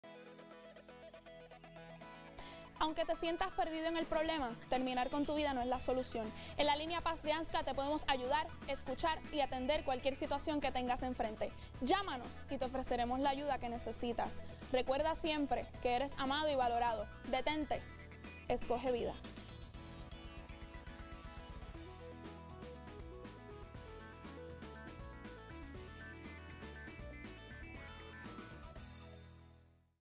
Selectos en la Comunidad y Marileyda Hernández se unen a ASSMCA
Selectos en la Comunidad y Marileyda Hernández se unen a ASSMCA con la campaña de prevención del suicidio: Escoge Vida.
“Valora, Respeta y Ama Tu Vida” Si tú o alguien que conoces necesita ayuda, llama a la línea PAS al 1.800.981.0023.